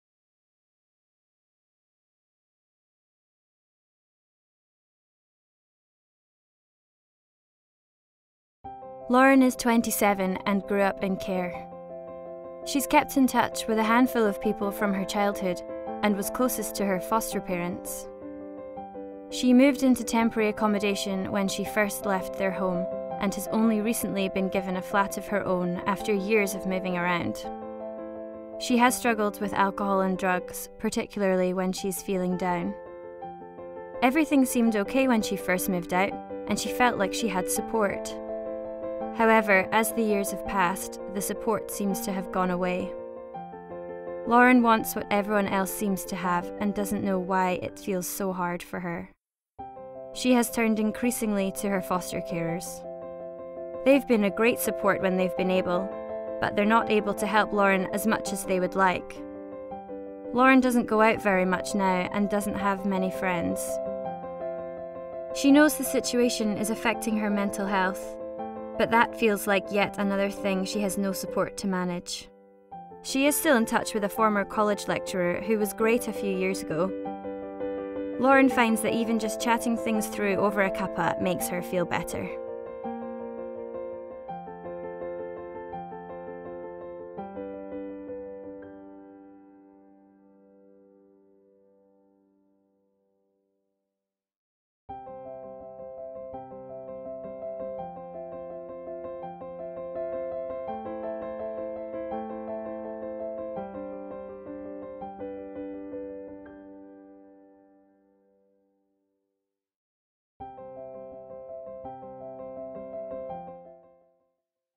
Lauren's story before the promise is kept
Lauren's story is made up of the real stories of care-experienced children across Scotland. It shows some of the challenges they face in "the care system" as it exists now.

But Scotland has made a promise to children with experiences like Lauren's, to make sure they grow up loved, safe and respected.